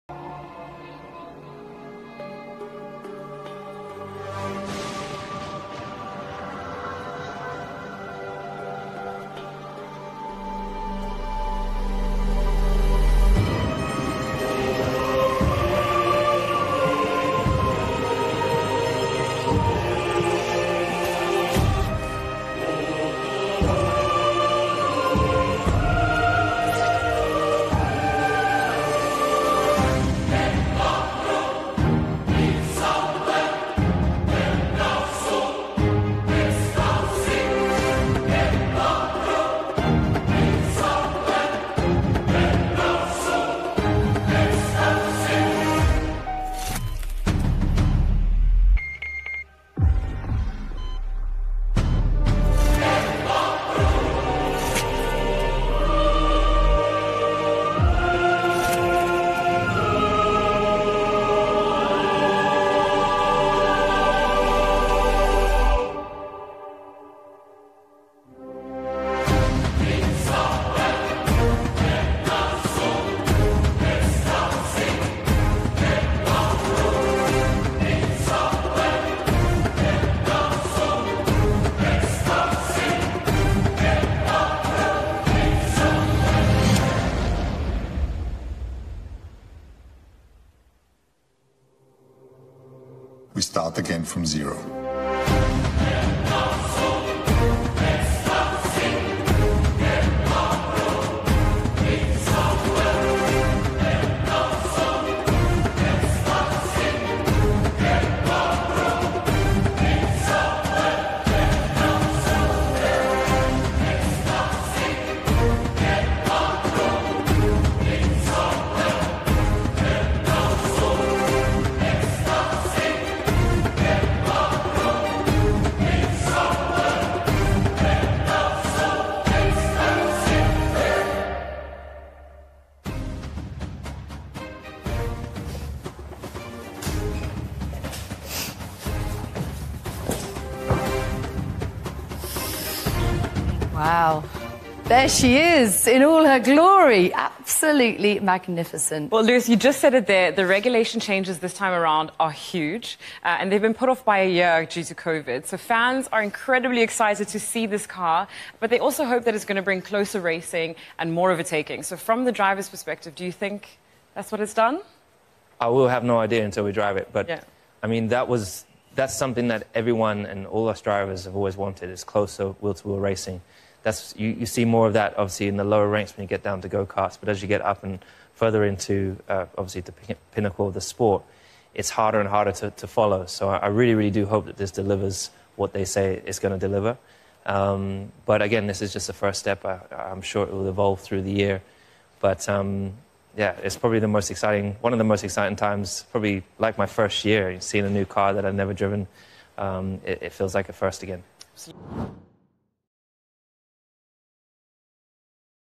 Lichtshow und epische Musik: Mit DIESEM Mercedes will Hamilton wieder Weltmeister werden!
Das Mercedes-AMG Formel-1-Team hat mit einer spektakulären Lichtshow und filmisch anmutender Musik seinen Rennwagen für die Saison 2022 vorgestellt. Lewis Hamilton hofft aufgrund der Regeländerungen auf mehr Überholmanöver und spannendere Rennen.